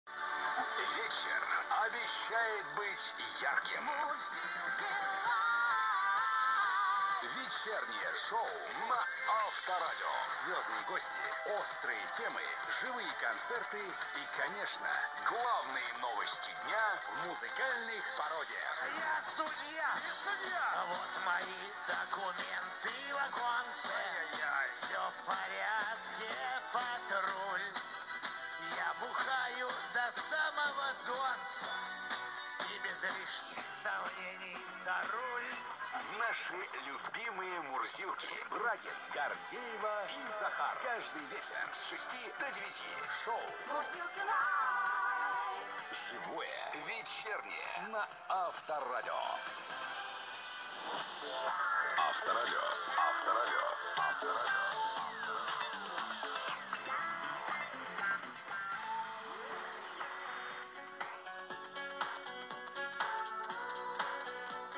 88.30 MHz Avtoradio (Авторадио). Reception of Russian FM radio in Belarus
Attention! Before viewing, increase the volume!

88.30 MHz Avtoradio (Авторадио). Reception of Russian FM radio in Belarus.

Reception from Dudarevo, Dobrush District of Belarus. 15 kilometers to Gomel, Belarus; 55 km to Novozybkov, Russia; 29 to the border BY-RU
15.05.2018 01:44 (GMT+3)
Degen DE1103 (DSP) telescopic antenna